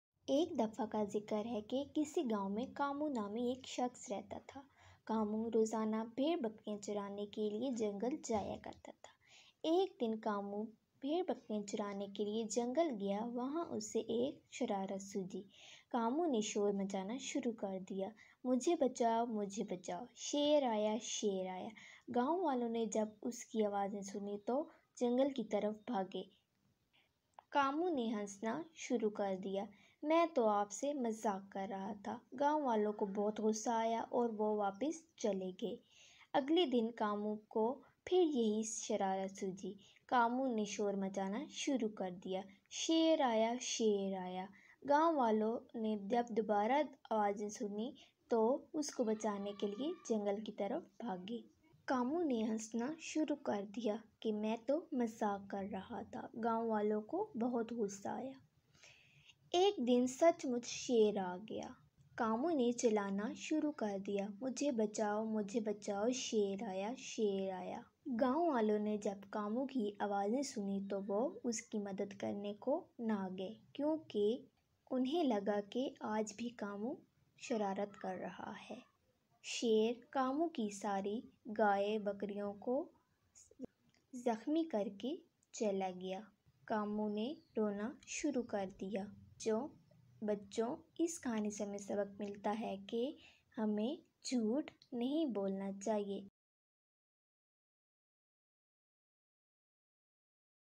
cartoon Moral story||jhoot ki SZA||جھوٹ کی سزا
is vedio m bachon ko sekhyea gyea h k k humen jhoot nhi bolna chiye.
this vedio is about moral story.
jhoot ki szaaa